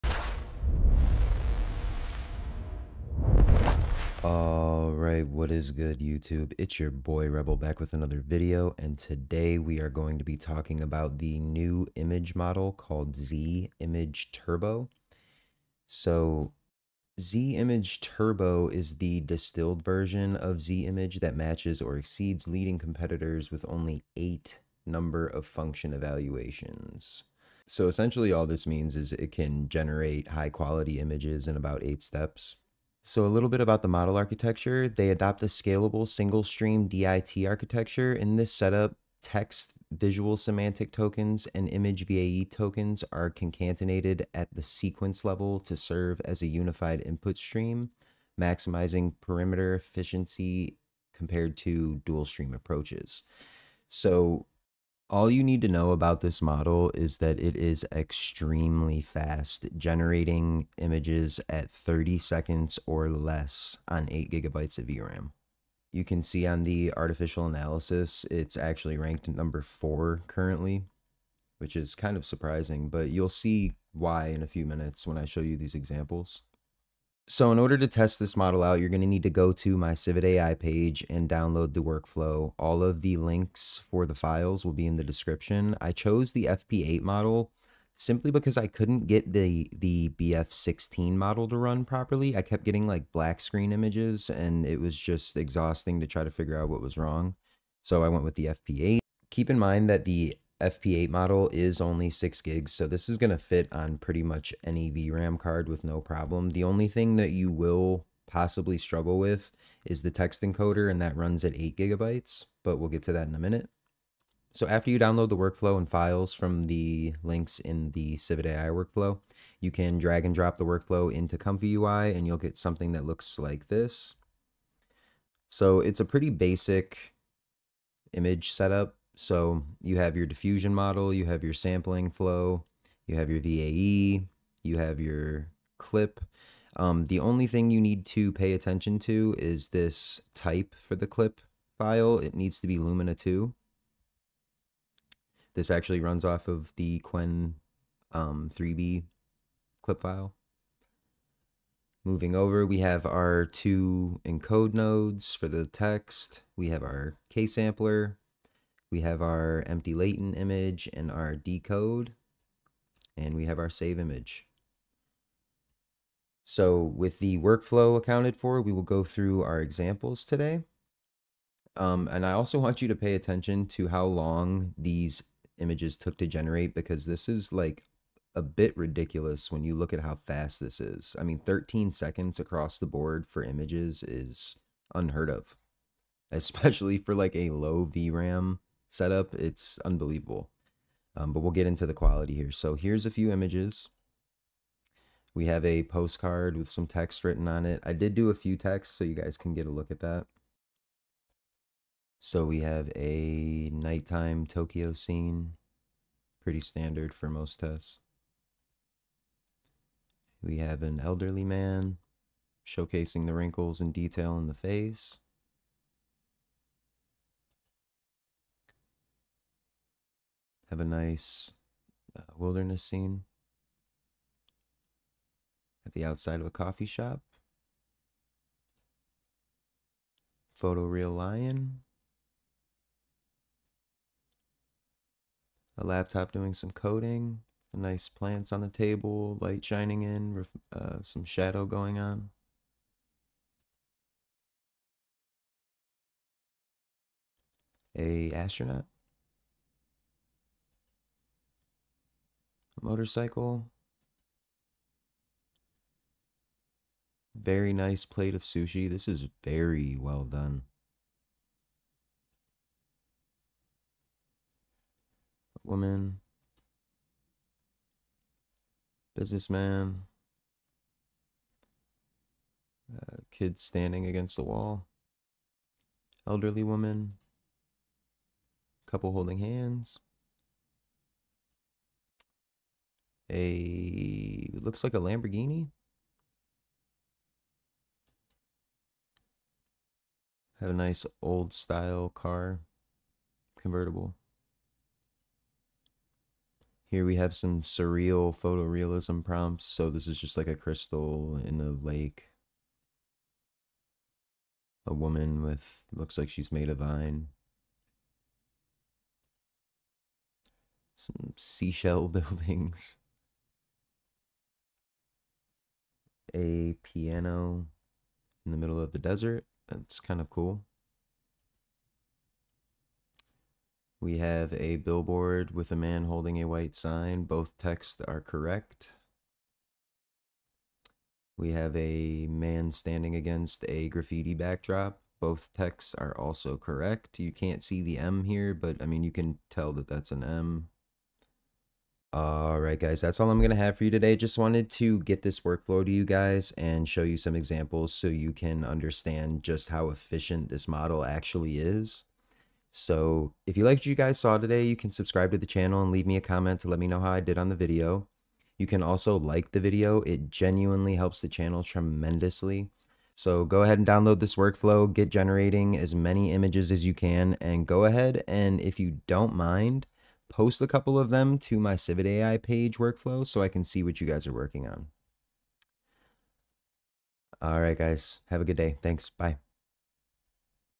Z-Image-Turbo For LOW VRAM | Workflow Tutorial
❗️UPDATE TO THE LATEST VERSION OF COMFYUI❗️
MODEL FILES ARE LINKED IN THE WORKFLOW⬇️

(Version 2 workflow includes a prompt cycler to generate images on autopilot. My newest video on my youtube page showcases how to use it in an in-depth tutorial! ❤)
Link to Prompt Cycler tutorial:

This is an amazing image model. i HIGHLY recommend testing it yourself. LOW VRAM USERS REJOICE ❤️

yes... its uncensored as well.